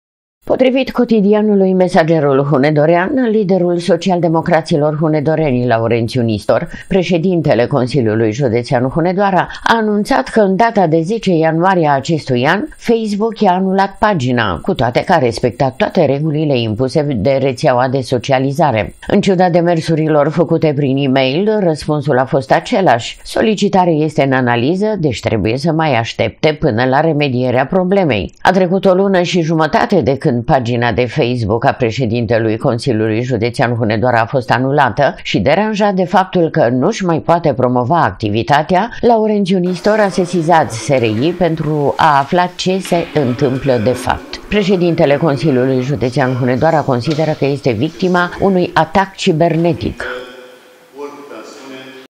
RĂMAS FĂRĂ PAGINA DE FACEBOOK, LAURENȚIU NISTOR A SESIZAT LA SRI UN ATAC CIBERNETIC
Potrivit cotidianului Mesagerul Hunedorean, liderul social-democraților hunedoreni Laurențiu Nistor, președintele CJ Hunedoara, a anunțat că în data de 10 ianuarie a acestui an, FB i-a anulat pagina, cu toate că a respectat toate regulile impuse de rețeaua de socializare. În ciuda demersurile făcute prin email, răspunsul a fost același: solicitarea este în analiză, deci trebuie să mai aștepte până la remedierea problemei. A trecut o lună și jumătate de când pagina de FB a președintelui CJ Hunedoara a fost anulată, și deranjat de faptul că nu-și mai poate promova activitatea, Laurențiu Nistor a sesizat SRI pentru a afla ce se întâmplă de fapt. Președintele CJ Hunedoara consideră că este victima unui atac cibernetic.